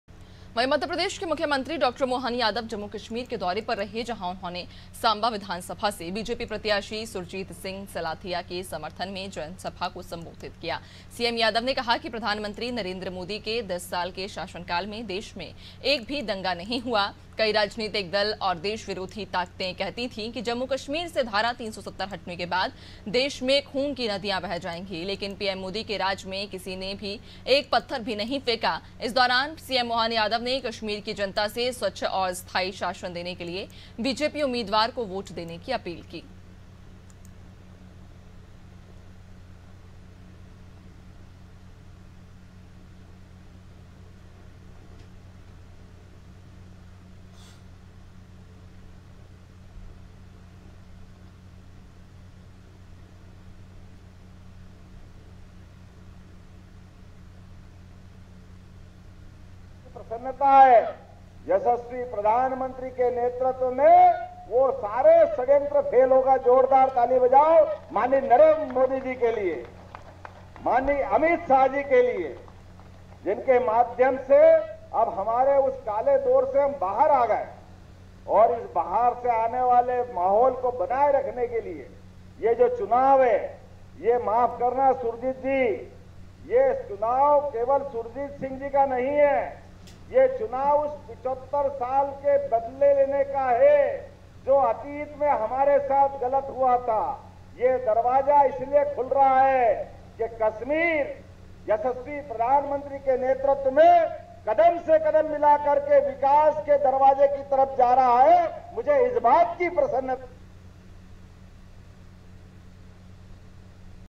Jammu Kashmir : सीएम डॉ. मोहन यादव का दौरा, रामलीला मैदान में की जनसभा || Anaadi Tv
मध्यप्रदेश के मुख्यमंत्री डॉक्टर मोहन यादव जम्मू-कश्मीर दौरे पर रहे...जहां उन्होंने सांबा विधानसभा से बीजेपी प्रत्याशी सुरजीत सिंह सलाथिया के समर्थन में जनसभा को संबोधित किया...सीएम यादव ने कहा कि प्रधानमंत्री नरेन्द्र मोदी के दस साल के शासनकाल में देश में एक भी दंगा नहीं हुआ...कई राजनीतिक दल और देश विरोधी ताकतें कहती थीं कि... जम्मू-कश्मीर से धारा 370 हटने के बाद देश में खून की नदियां बह जाएंगी... लेकिन पीएम मोदी के राज में किसी ने एक भी पत्थर नहीं फेंका...इस दौरान सीएम मोहन यादव ने कश्मीर की जनता से स्वच्छ और स्थायी शासन देने के लिए बीजेपी उम्मीदवार को वोट देने की अपील की।

AIRTEL - 379